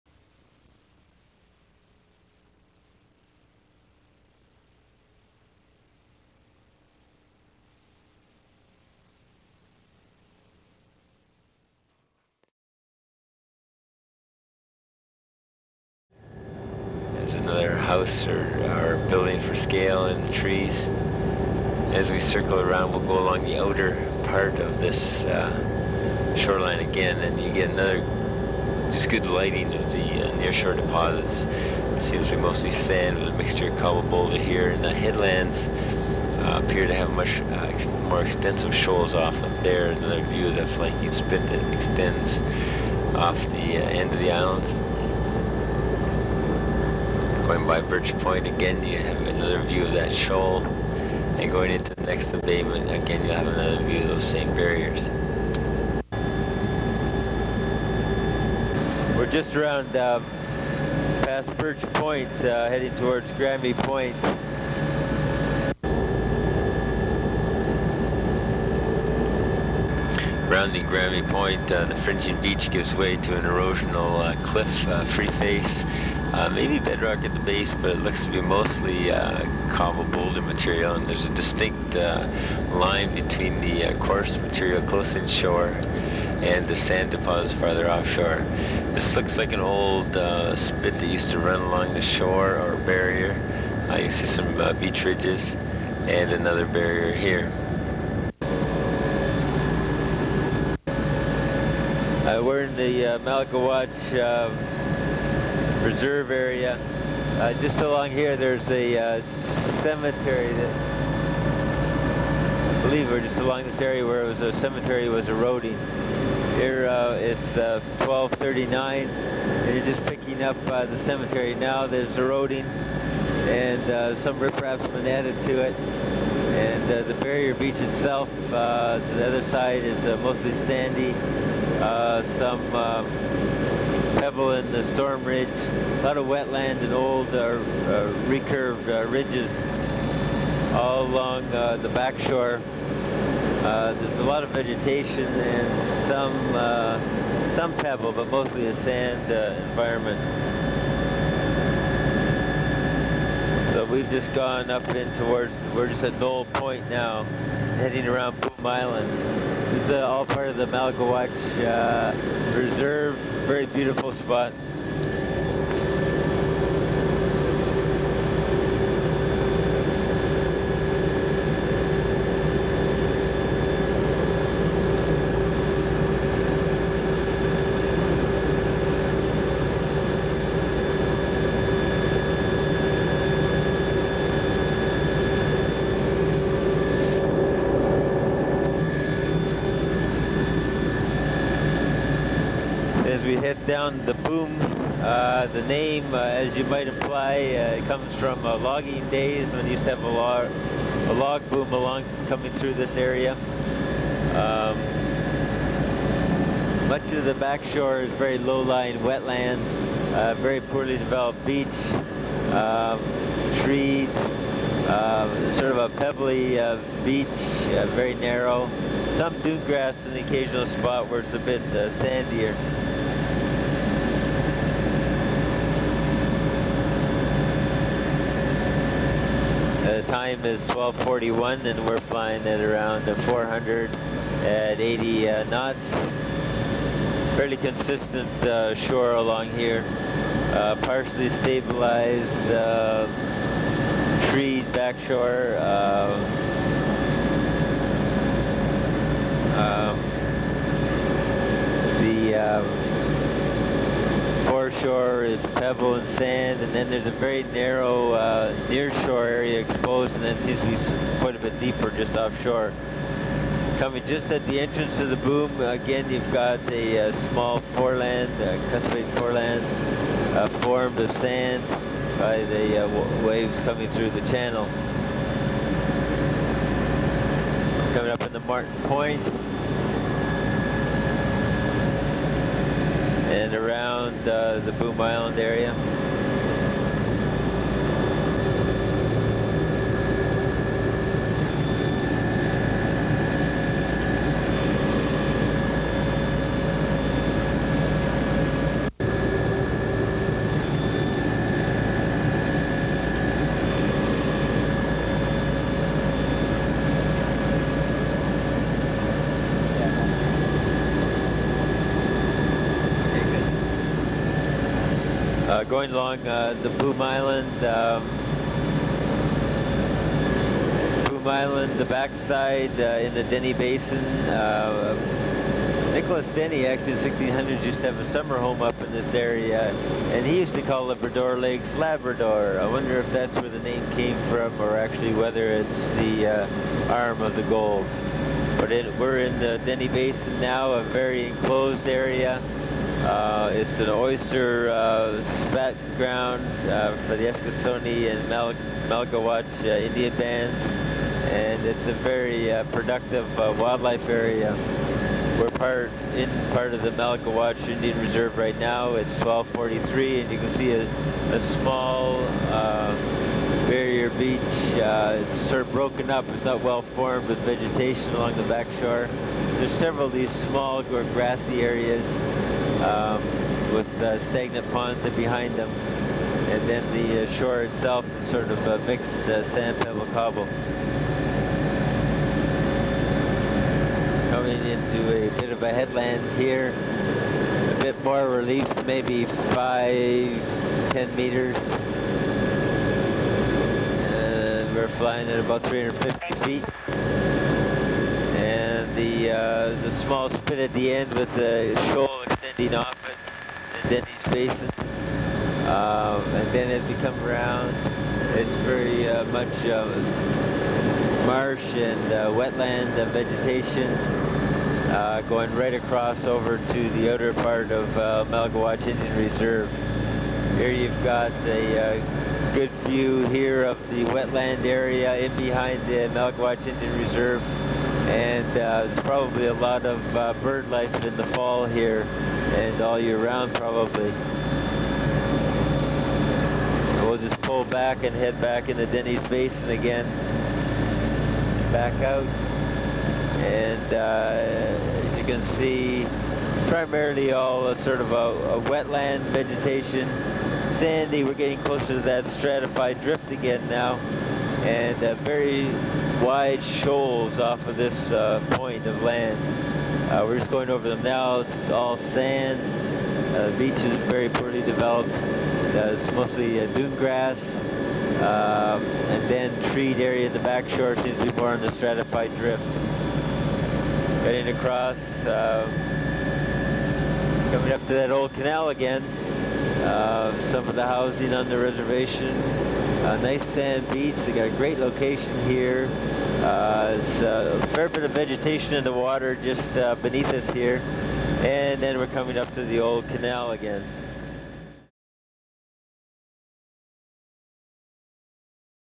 Bras D'Or Lake Aerial Survey - Part 2 of 7
This is the second in a series of aerial survey videos of the Bras D'Or Lake shoreline that were made by the Canadian Geological Survey in June of 1996. Each video posted to YouTube has to be less than 10 minutes and so the total of 57 minutes has been divided up into 7 separate segments. This video covers the shoreline from XXXX to XXXX and was recorded on 24th June, 1996.